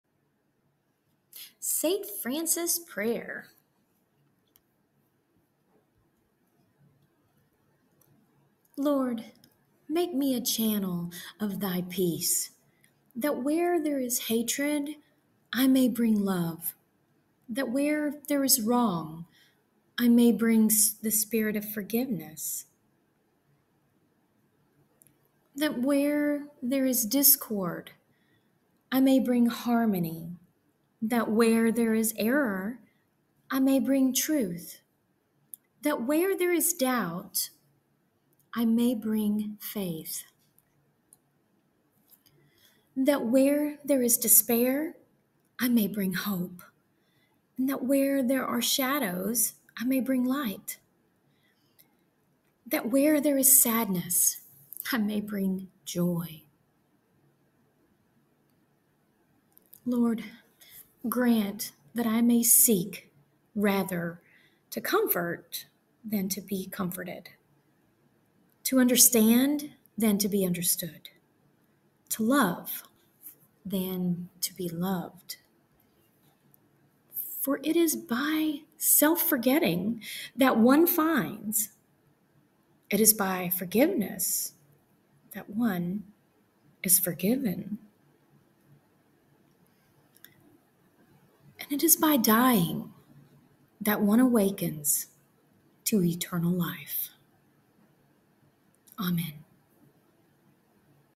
St Francis Prayer
St Francis Prayer read by Chrissy Polovich of Mountains to the Sea Ministries Inc at Wellspring Healing & Prayer Retreat.

We are passionate about recovery. If you know someone who needs help, please reach out! There is hope.